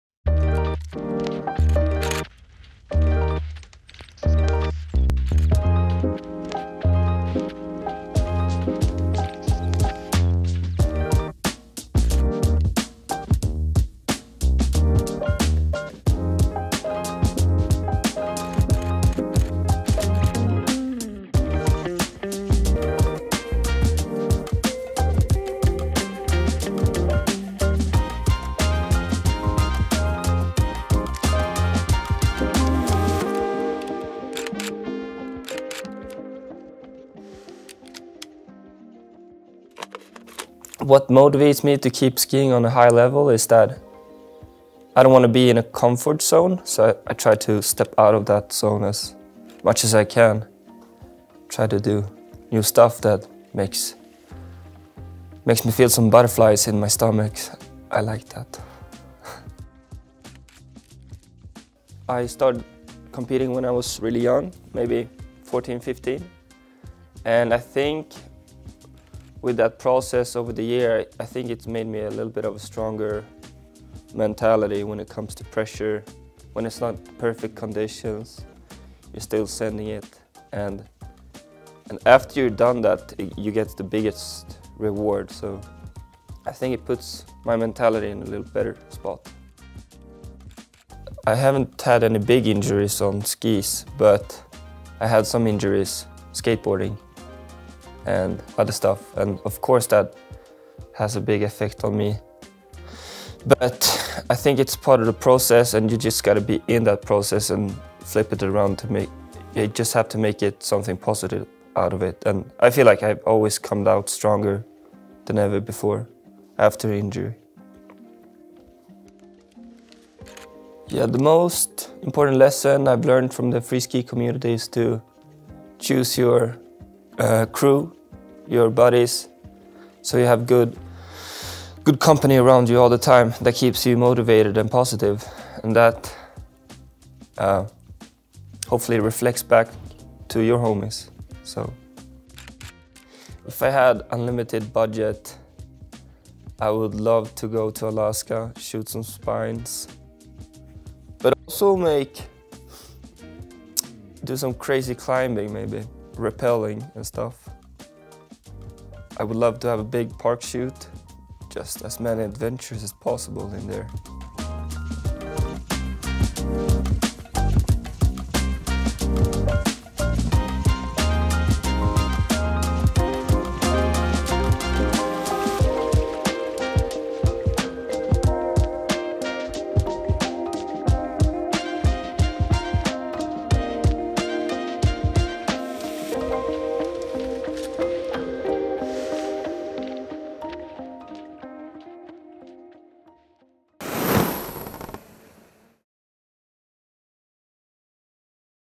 Emil Granbom
Emil Granbom has recently transitioned from the World Cup stage to the world of ski movies. What is the most important lesson he has learned from the freeski community?
Discover this and much more in this new episode of our keepsyouskiing series.